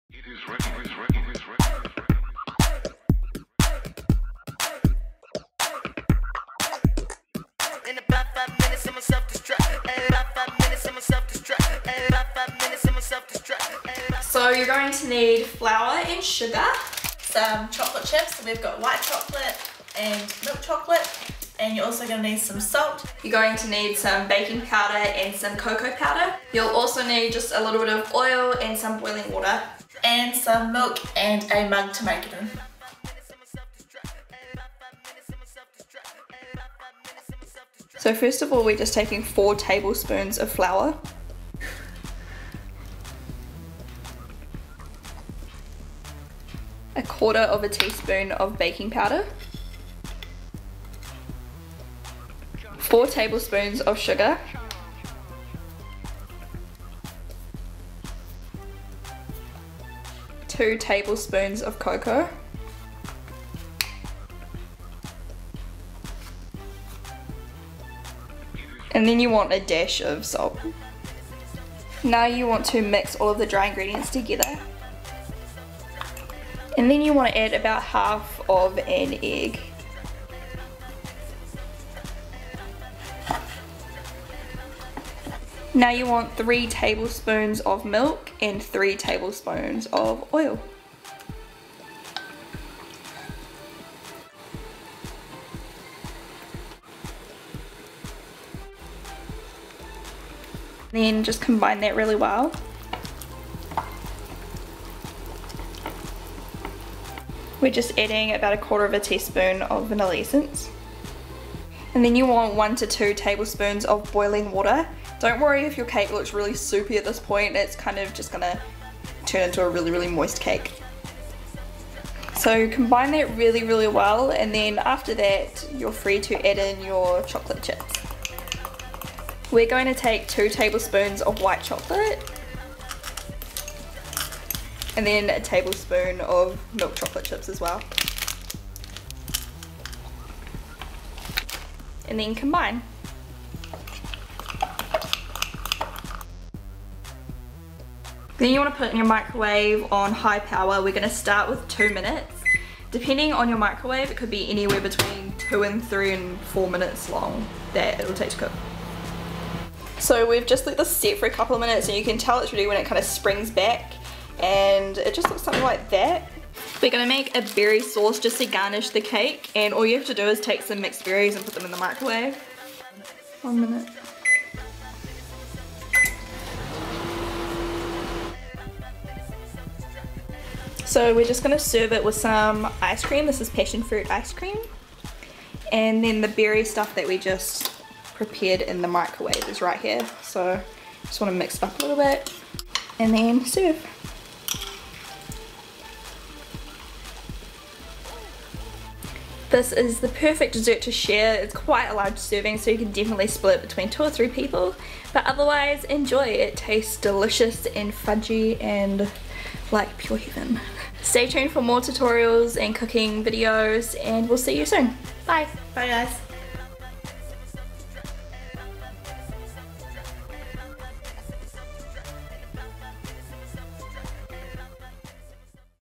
♡ 2 Minute Chocolate Fudge Mug Cake ♡ Microwave Recipe
What You Need & Ingredients:
4T Flour
4T Sugar
1/4t Baking Powder
Dash of Salt
3T Cooking Oil
1 or 2T Boiling Water
1/4t Vanilla Essence
2T Cocoa Powder
3T Milk
1/2 Egg
3T Chocolate Chips

Method:
Mix together dry ingredients
Add egg and combine
Add milk and oil, combine
Add vanilla, and boiling water. Combine
Add chocolate chips.
Microwave on high for 2 minutes, or until cake springs back

Optional: Serve with icecream and microwaved berries

Leave your requests down below!